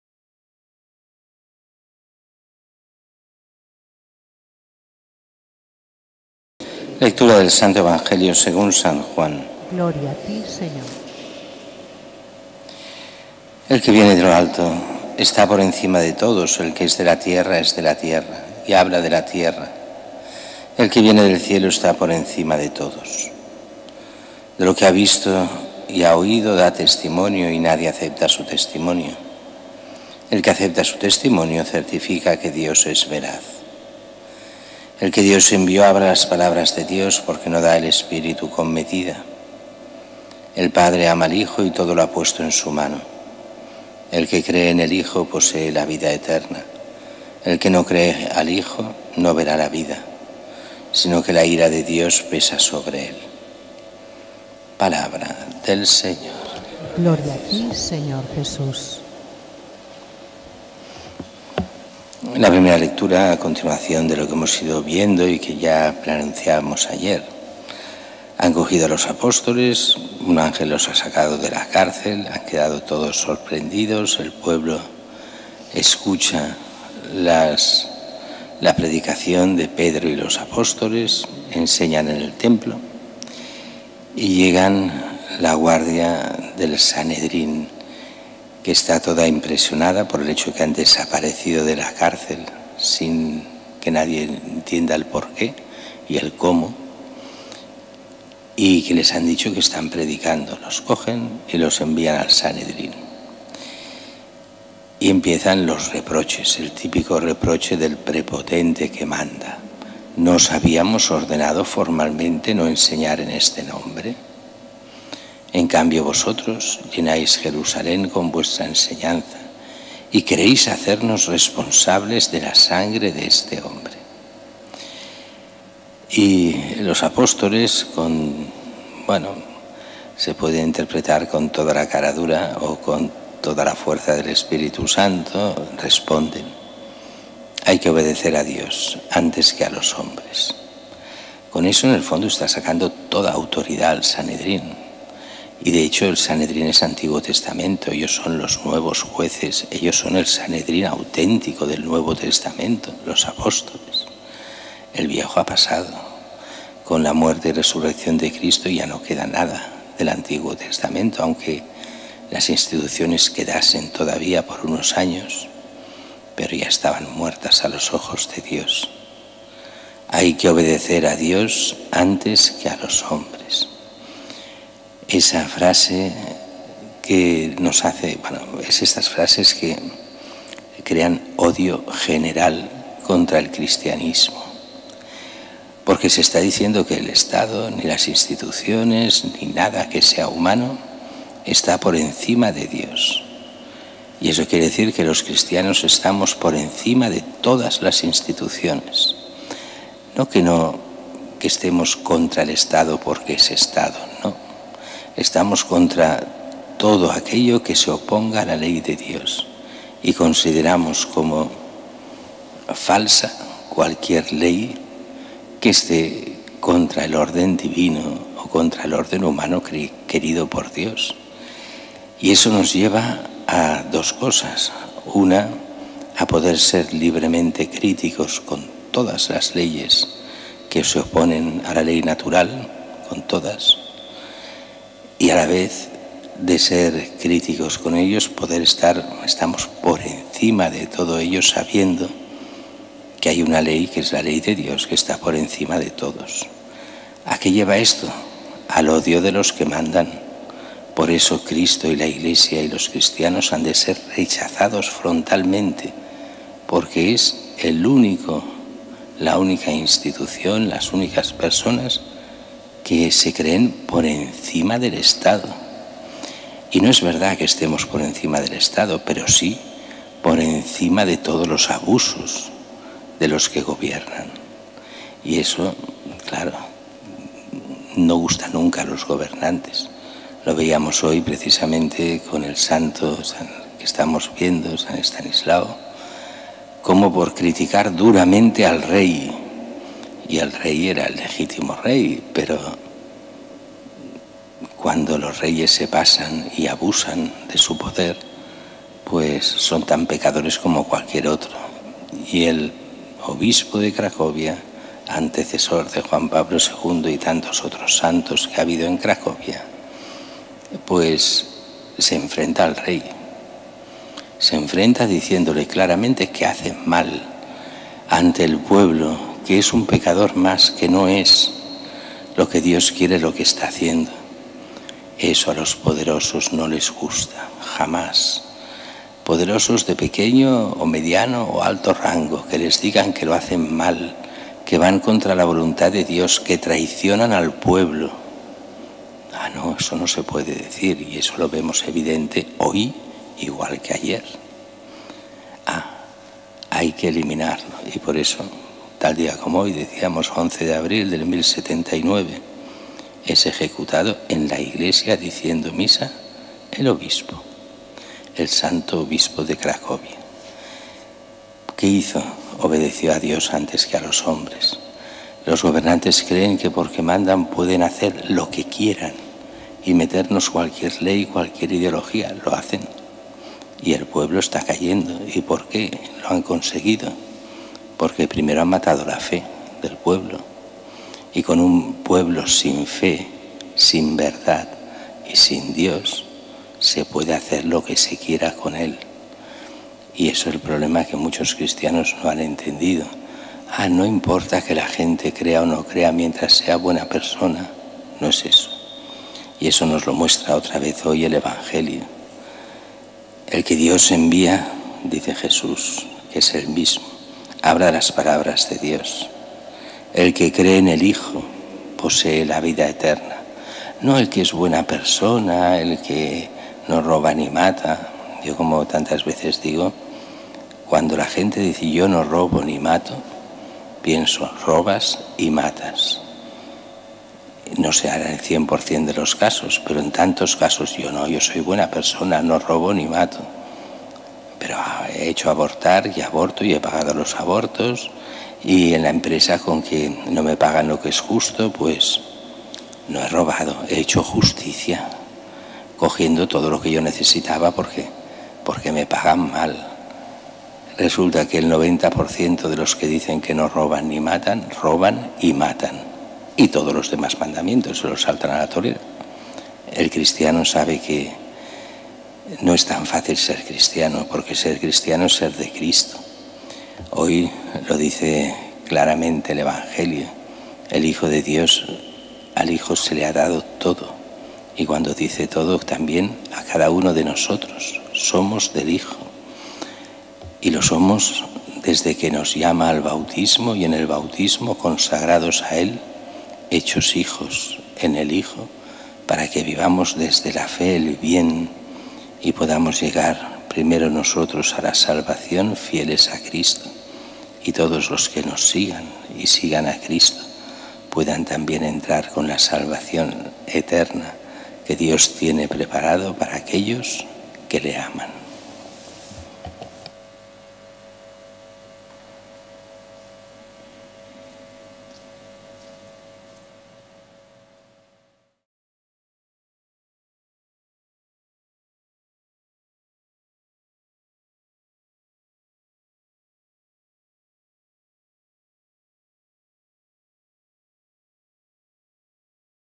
Evangelio y homilía de hoy jueves 11 de abril de 2024. Rvdo. Enric Ribas, pbro.
→ Nazaret TV te ofrece el evangelio y la homilía de la Santa Misa del día de hoy jueves 11 de abril de 2024.
Predica: Rvdo. Enric Ribas, pbro.
Desde la parroquia de Santa Teresita del Niño Jesús, en Barcelona.
Vía Augusta 68 (Barcelona)
Día litúrgico: Jueves de la 2ª semana de Pascua.
El evangelio de hoy corresponde a san Juan 3,31-36